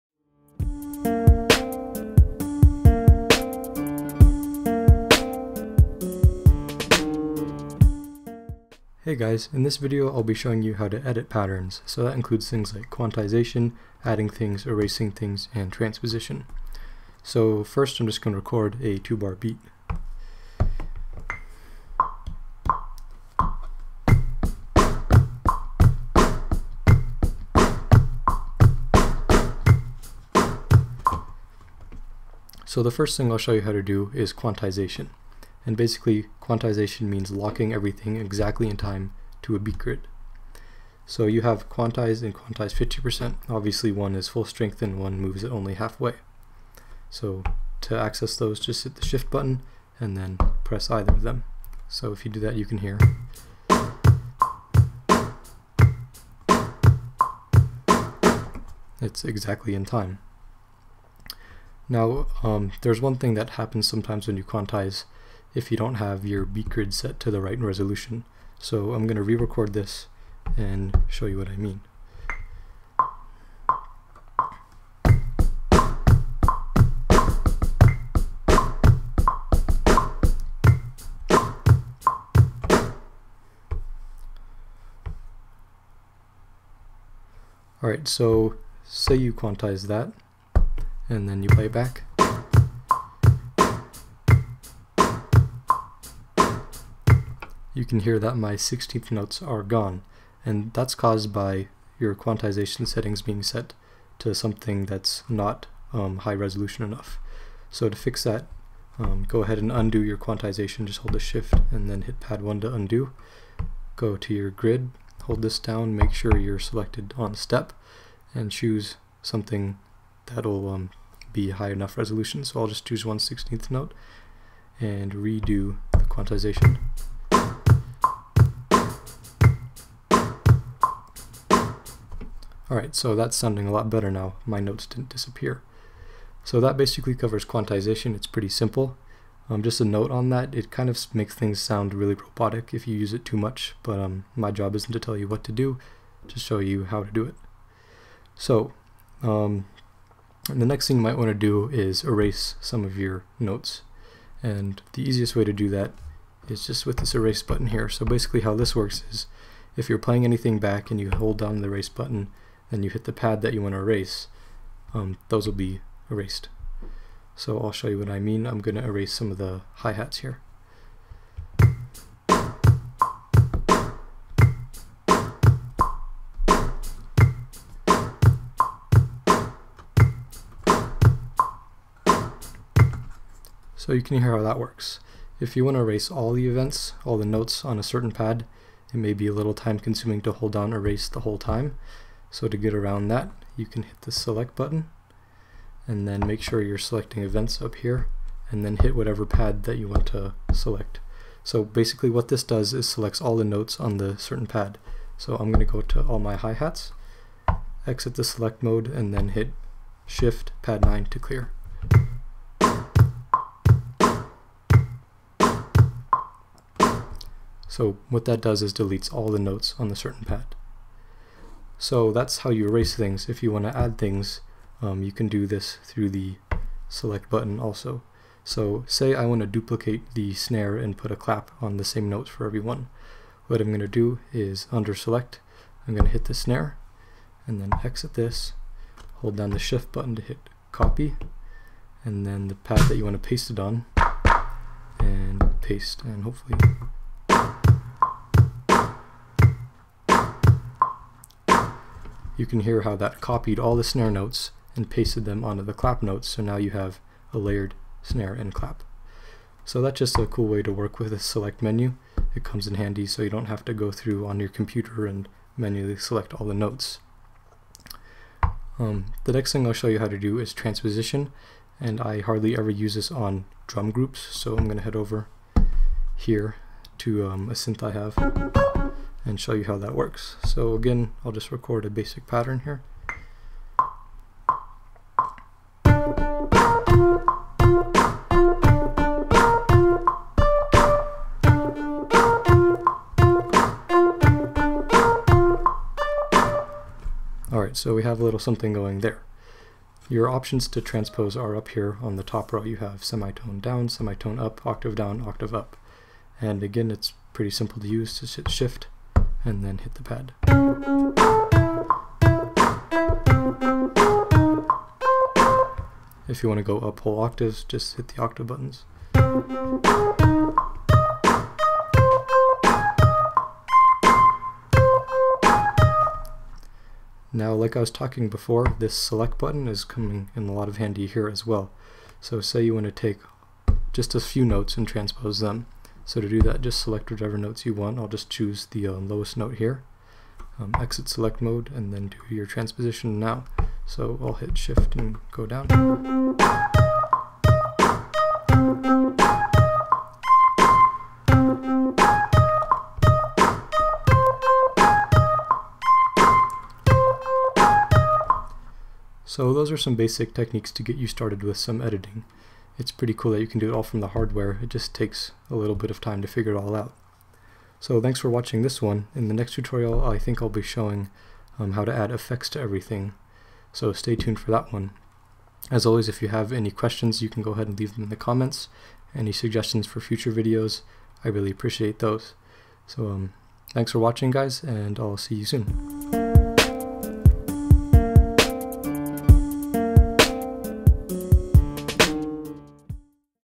Maschine Mikro Tutorial #3: Editing Recorded Patterns
How to edit recorded patterns on the Maschine Mikro, including things like quantization, erasing, and transposition.